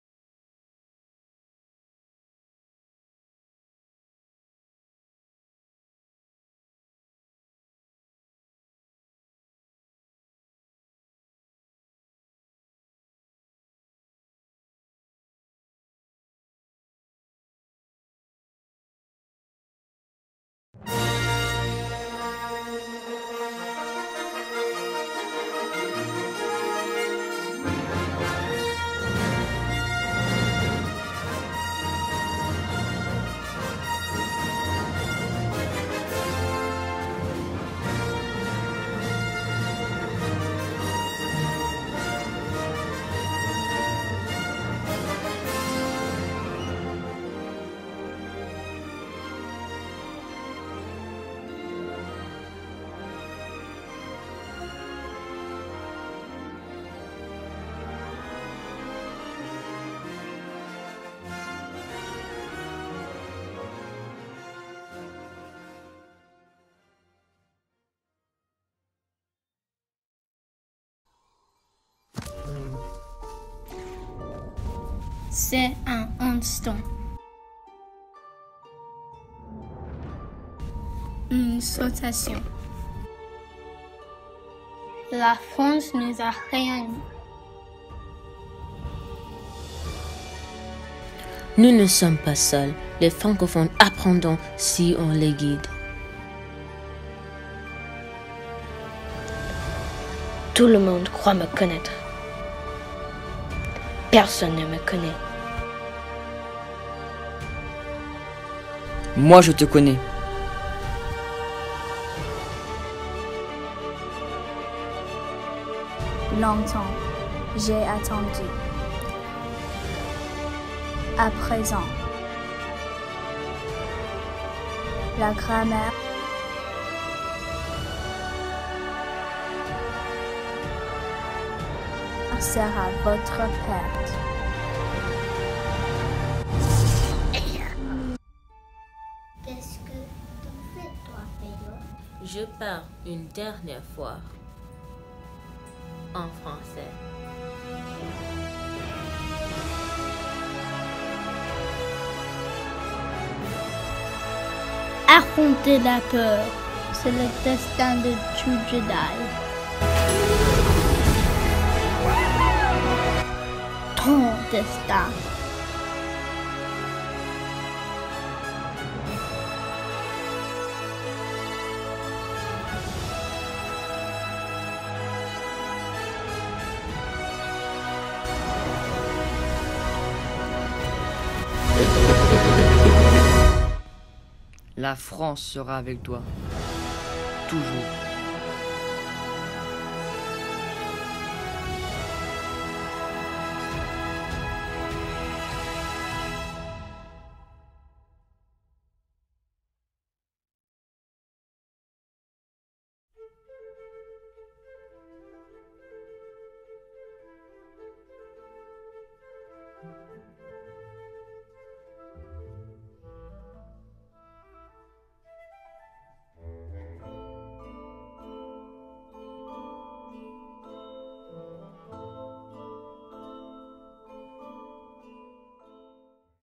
Star Wars: The French Awakens (Trailer by FASTB)
Bonjour! Here is the Star Wars trailer made my the students at FASTB to celebrate International Francophonie Day!